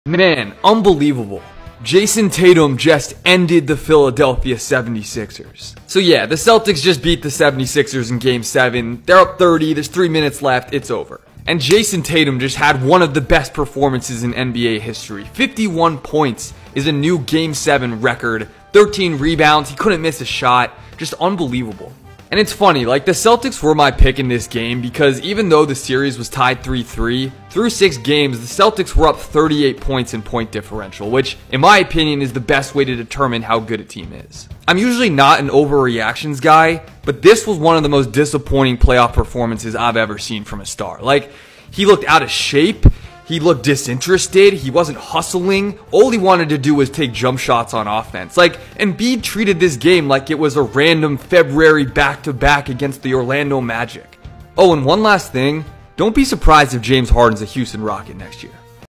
Jayson Tatum just ENDED the 76ers 💀
full reaction to Celtics vs 76ers Game 7. Jayson Tatum is HIM.